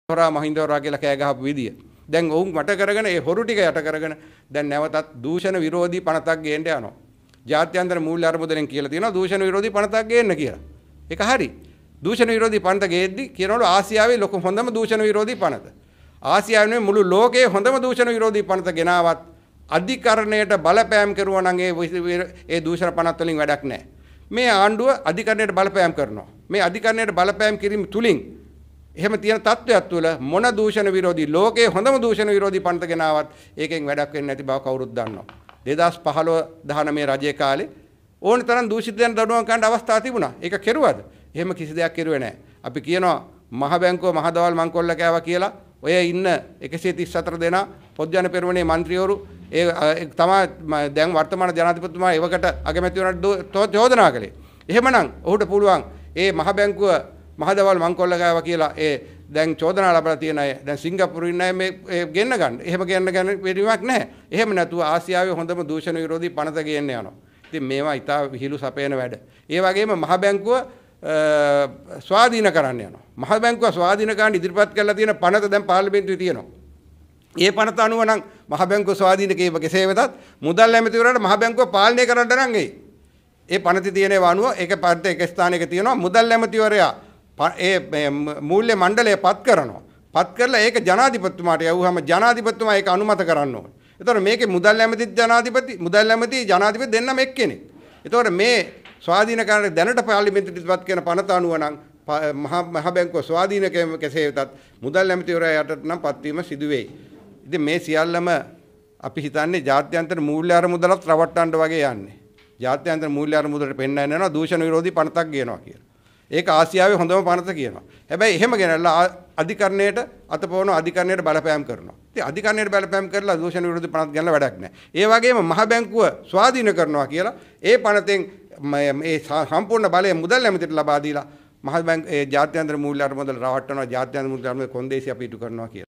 මුල්‍ය අරමුදලත් රවටන්නයි ආණ්ඩුව හදන්නේ..
හොඳ ම දූෂණවිරෝධී පනත හැදුවත් වැඩක් තියෙනවා ද අධිකරණයට බලපෑම් කරන ආණ්ඩුවක්?